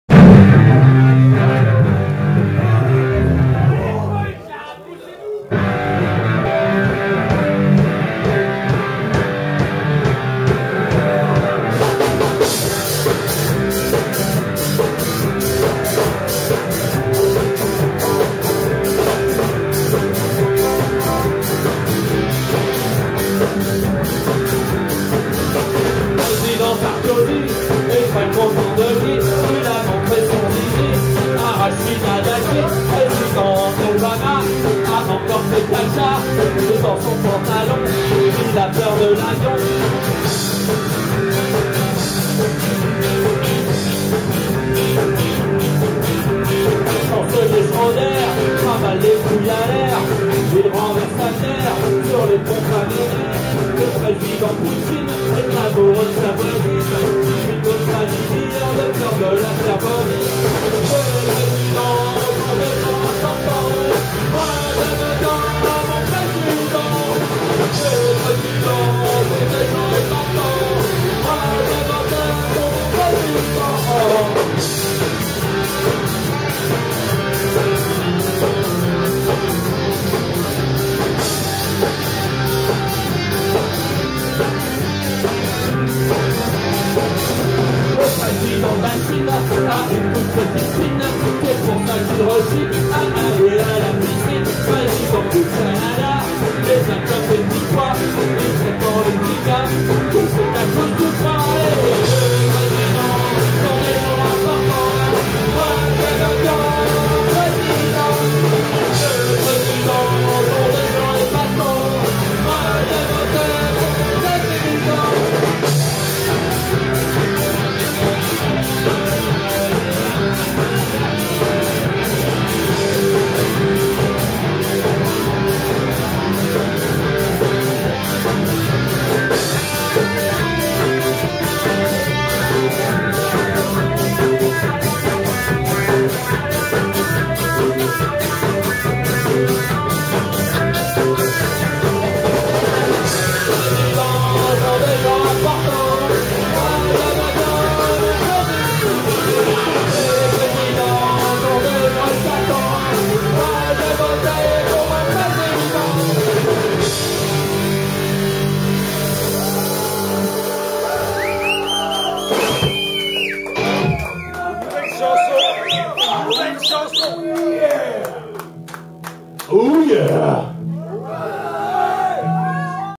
les presidents Nono Futur - live at manhattan cafe melun
DRUNKER fête ses 3 ans, alors venez foutre avec nous un putain de bordel! Nos potes de MATOS DE MERDE seront de la partie et enfin, cerise sur le gâteau, NONO FUTUR viendra compléter cette soirée ! DRUNKER aura le plaisir de vous faire découvrir son nouveau line up ! Un chose est sûre ça va envoyer sévère !
au manhattan cafe 24 bld victor hugo 77000 melun